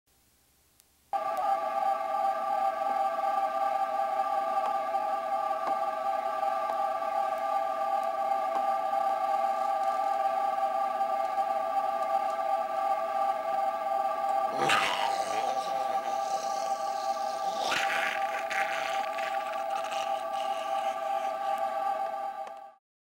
Unlabeled VHS I received from Luke
I got a manila envelope with no return address in the mail a few days ago.  Inside was an unlabeled vhs tape.  It was video of a person which after watching for a few seconds I recognized as a friend of mine I used to hang out with a few years ago, named Luke.  I haven't talked to him in a couple of years because we live on opposite coasts.  I don't know... its kind of screwed up.